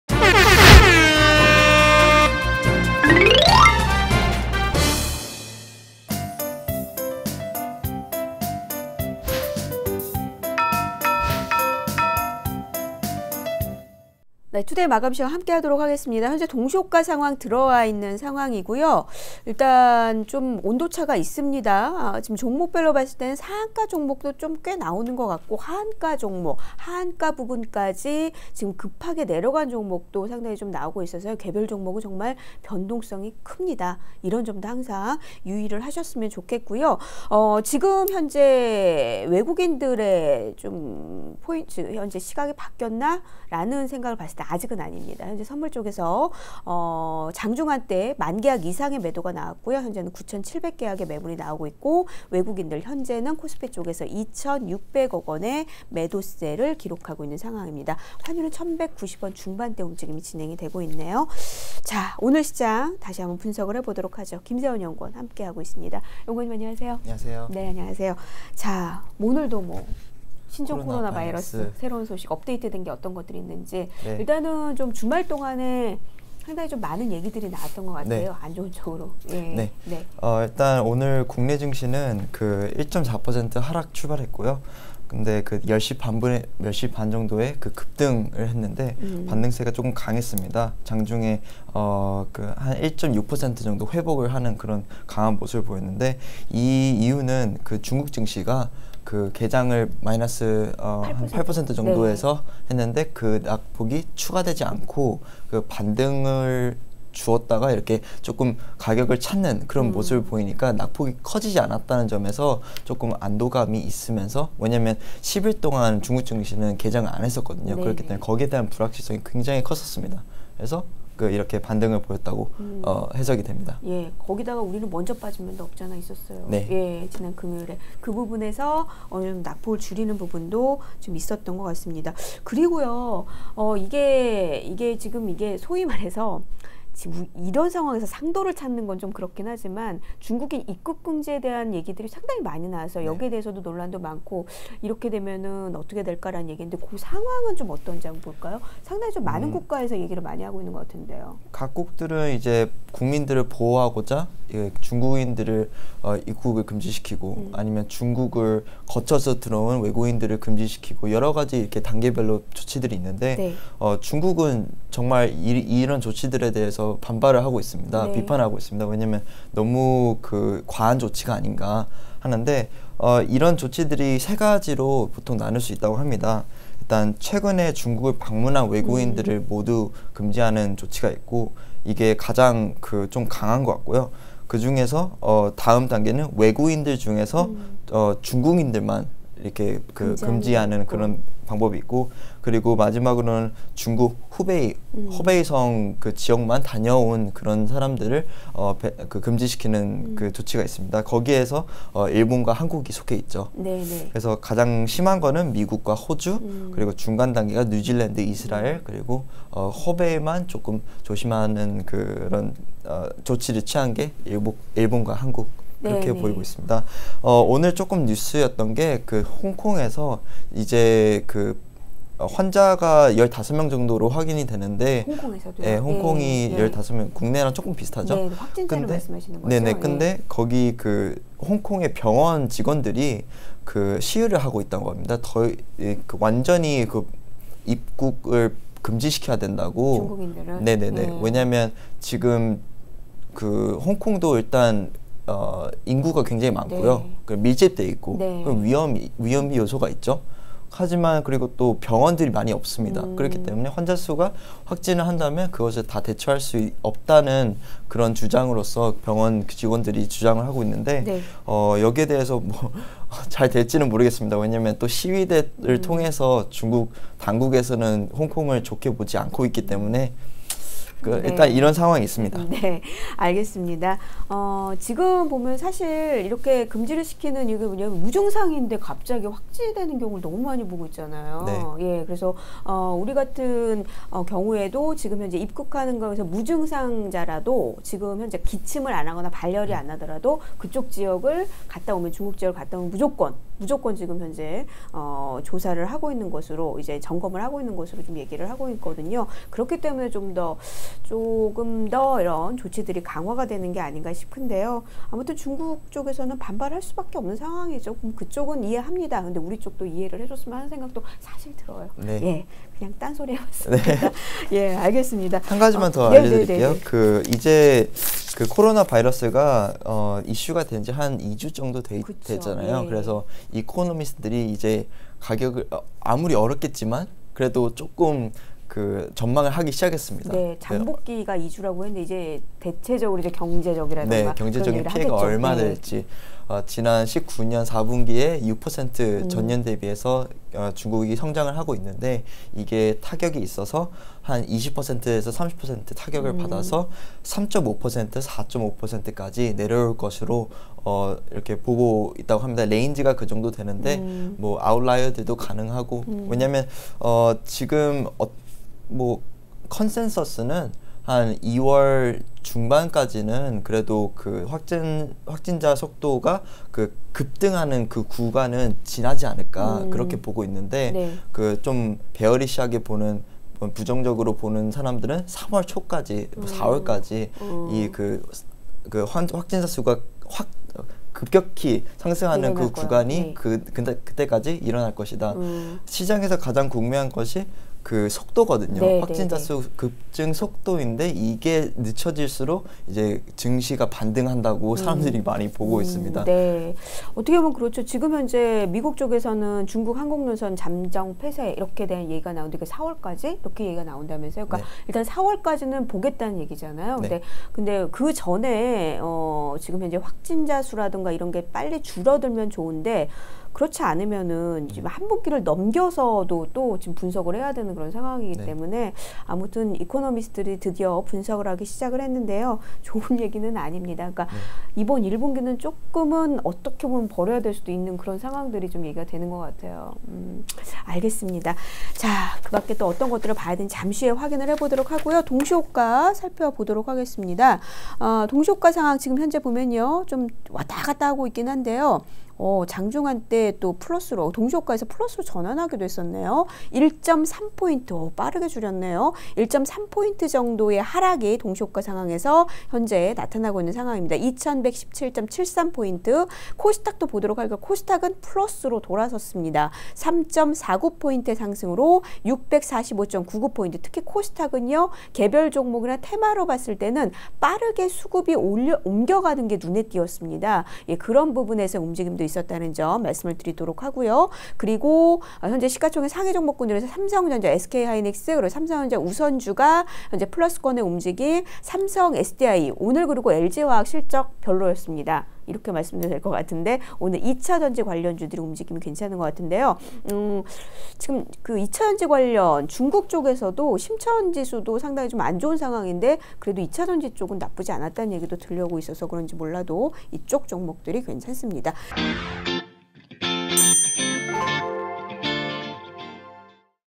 [주식투자][시장분석] 마감시황 / 중국 10거래일만에 개장, 국내증시 낙폭 축소한 이유는?(20.02.03)
중국 증시가 춘절 연휴를 끝내고 10거래일만에 개장(약 -8.7%대로 장 출발)
중국 증시 개장 이후 국내증시는 낙폭을 축소하는 모습
신종 코로나바이러스 관련 글로벌 소식으로는
홍콩병원에서 일하는 직원들이 중국인 입국금지 요구하며 시위
"더 심각한 상황이 발생하면 통제가 불가능할 것"이라고 주장
일부 주요 이코노미스트는 중국 경제적 피해에 대해 코멘트를 했으며
중국 올해 1분기 경제성장률 3.5~4.5%로 예상
오늘 국내증시에선
코스피에서 외국인이 3,073억 순매도, 기관 1,700억 순매수,
개인 1,177억 순매수 함​
김순영 키움증권 채널K 앵커, 김세헌 연구원 키움증권 투자전략팀